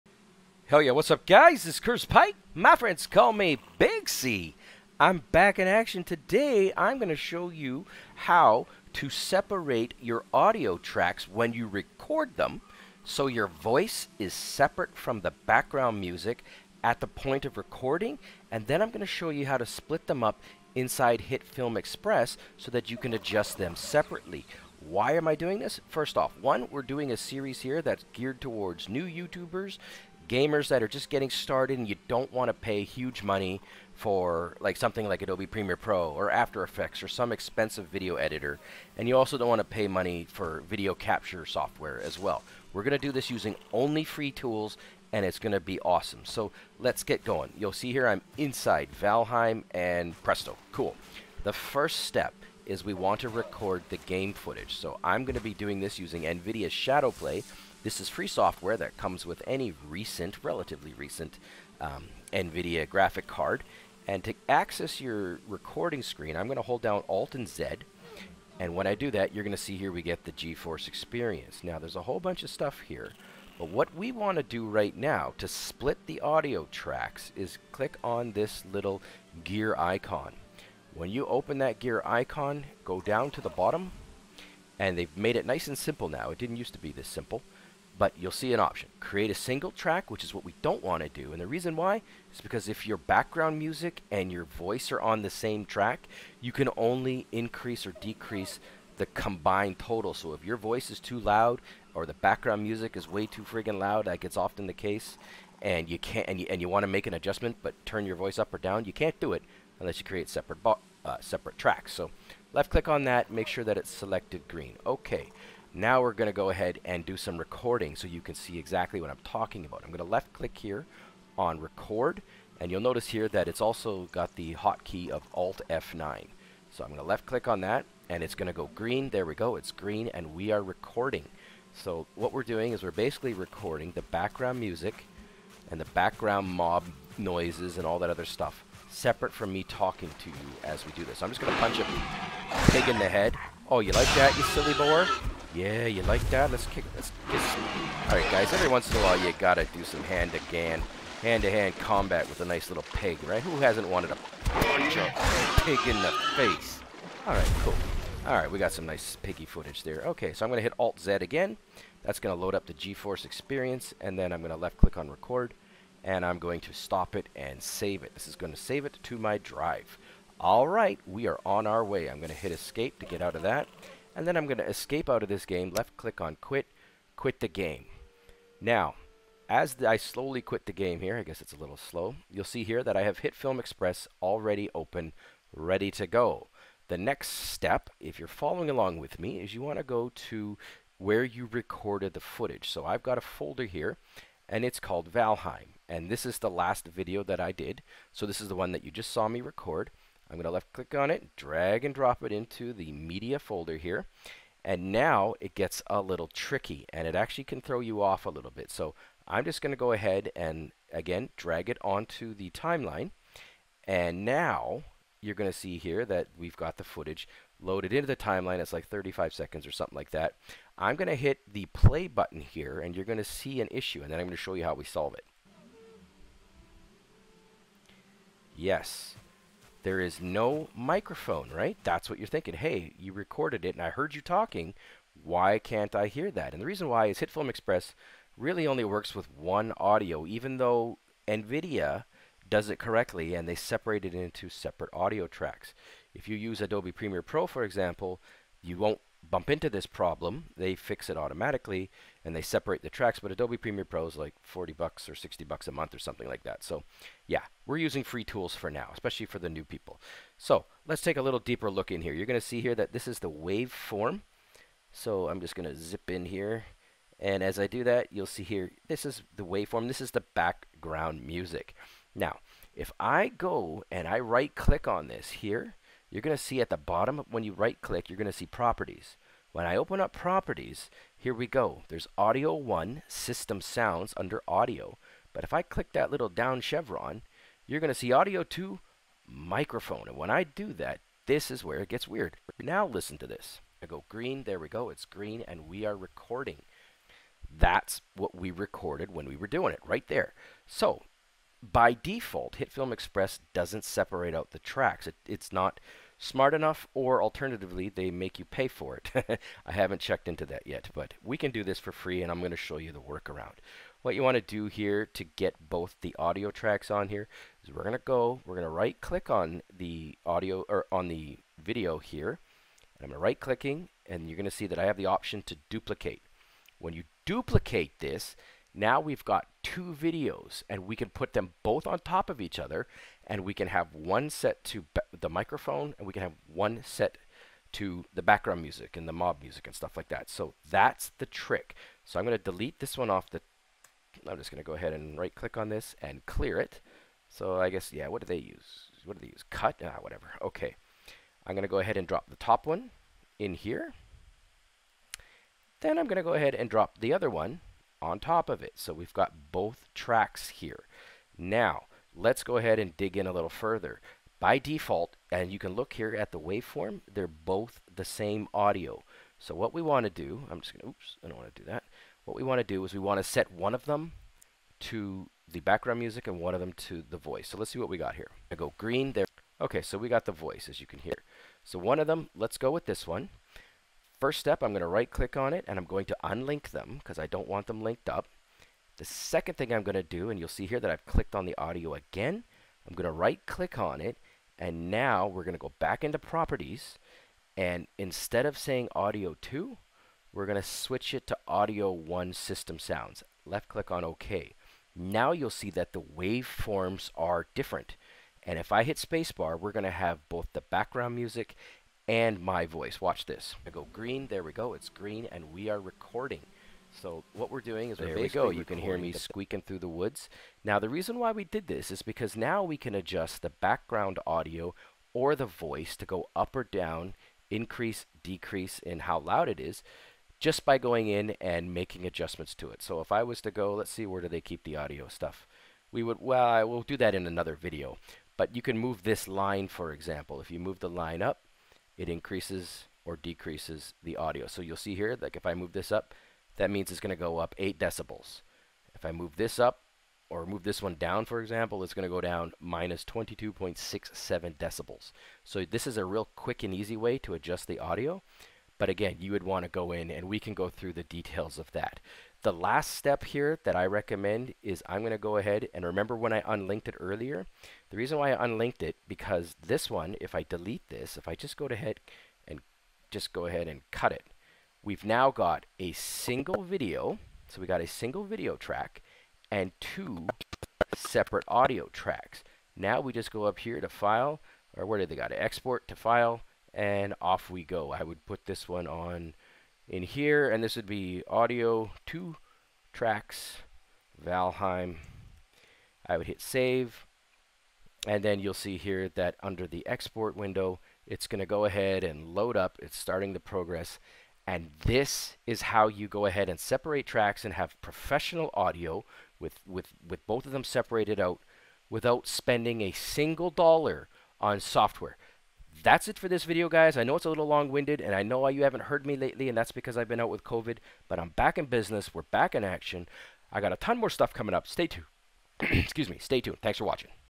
How To Record And Separate (Gaming) Audio Tracks Using FREE SOFTWARE
In this video Big C shows you how to record audio to separate tracks and combine them together using Free software - Nvidia Shadowplay and Hit Film Express.  The days of recording your voice with the background sounds of your game together are long gone.

First step is to load up the Nvidia GeForce Experience / Shadow Play.  Next, click on the gear icon and go down to audio.  Click on Separate tracks.

Now record yourself playing a video game using shadowplay (alt F9).  Drop the gameplay footage into the media folder.  Create a duplicate copy of the footage using duplicate.  Drop one track on video 1 / audio 1 and the second track on video 2 audio 2.  Now go to properties and for the first track select audio 1 - system sounds.  For the second track select audio 2 - Microphone.  Remove one of the videos from the timeline and then click on export!

▼ Chapters
0:00 -
0:30 -

That's it :-)